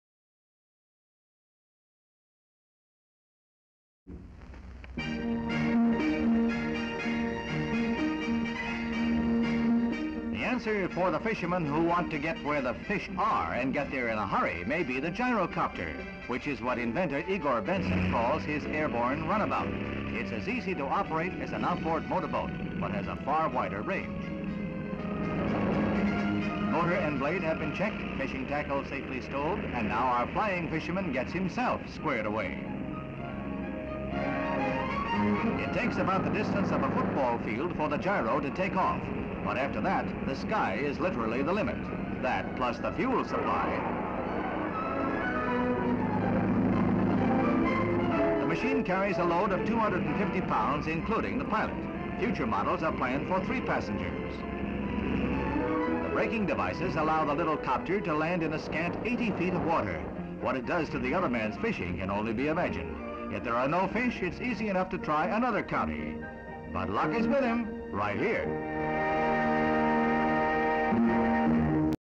A fisherman flying Gyro-copter. HD Stock Footage
A fisherman flying Gyro-copter.

A fisherman gets on to a helicopter which looks like a motorboat and also called as Gyro-copter. Gyro-copter running on water before it takes off. View of a fisherman flying Gyro-copter in sky. Gyro-copter lands in water. View of fisherman getting out of Gyro-copter. Location: Florida United States. Date: March 28, 1963.

57,000+ broadcast-quality historic clips for immediate download.